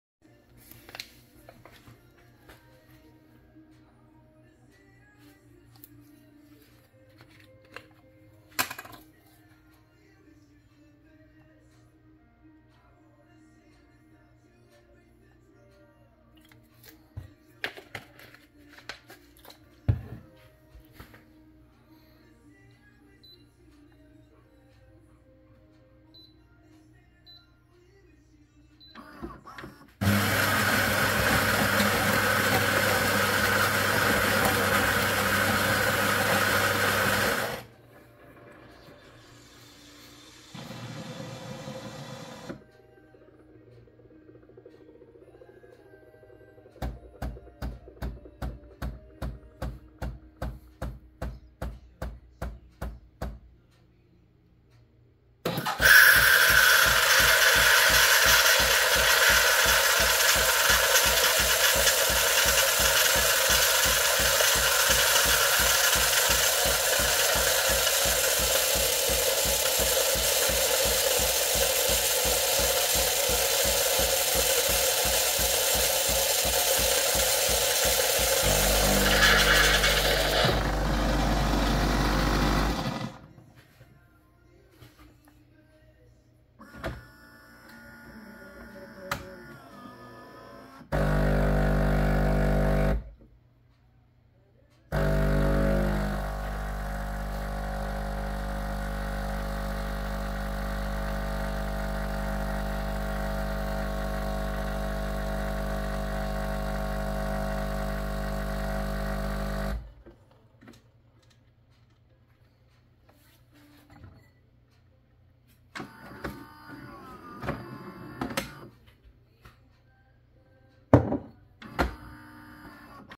Phillips LatteGo S5400 prepare small latte with Mullermilch pistachio 👌🇨🇿🇨🇿🇨🇿🇨🇿🇨🇿🇨🇿🇨🇿
Phillips LatteGo S5400